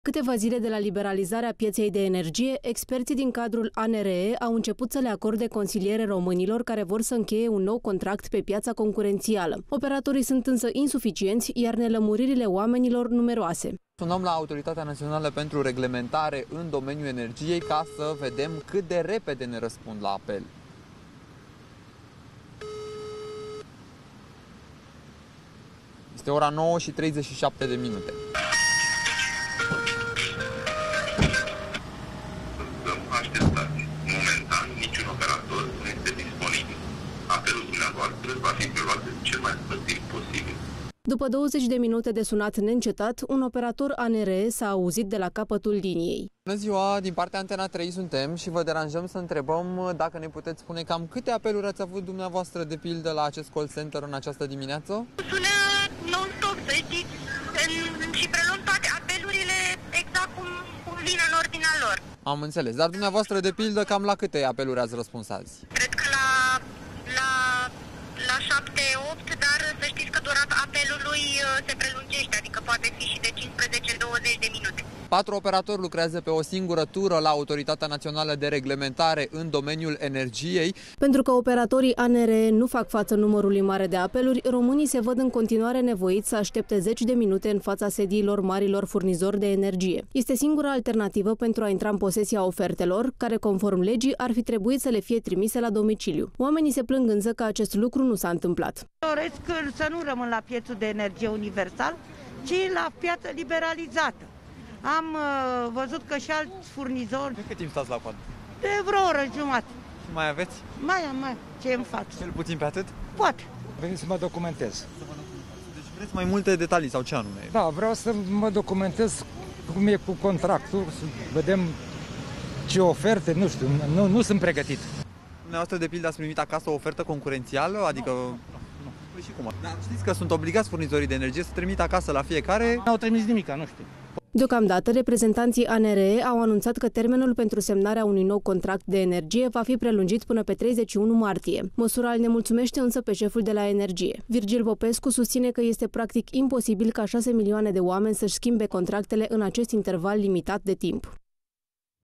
Reportaj despre call-center-ul înfiinţat de ANRE. Un reporter Antena 3 a sunat zeci de minute pân
Telefonul sună prea târziu la Autoritatea Naţională de Reglementare în Energiei (ANRE). Instituția a înființat un centru pentru cei care au nelămuriri care au legate de liberalizarea pieței, după ce procesul a avut loc. Un reporter Antena 3 a sunat zeci de minute în șir până a reușit să ia legătura cu operatoru ANRE. Cum eficiența cal-center-ului lasă de dorit, oamenii formează în continuare cozi în fața sediilor marilor furnizori de energie pentru a face rost de oferte.